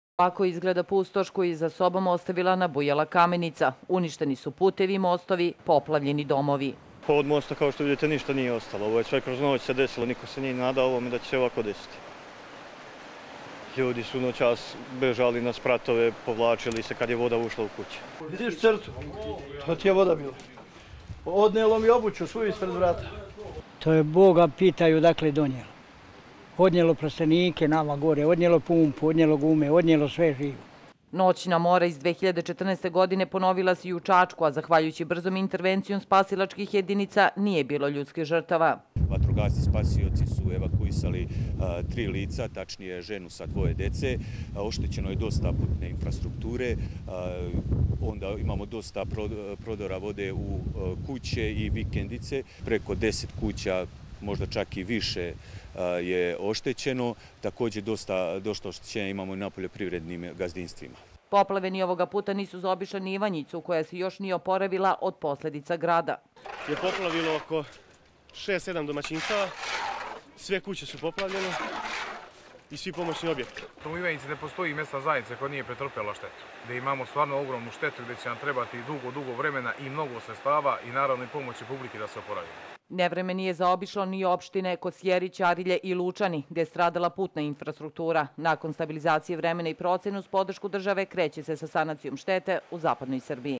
Momirović: Država će pomoći ugroženima od poplava i sanirati štetu
Ministar građevinarstva, saobraćaja i infrastrukture Tomislav Momirović obišao je danas uništenu putnu infrastrukturu i poplavljene kuće u opštini Gornji Milanovac, gde je usled vremenskih nepogoda došlo do izlivanja reke Kamenice i poručio da će Vlada Srbije uraditi sve da se ugroženima pruži pomoć, a šteta bude sanirana u rekordnom roku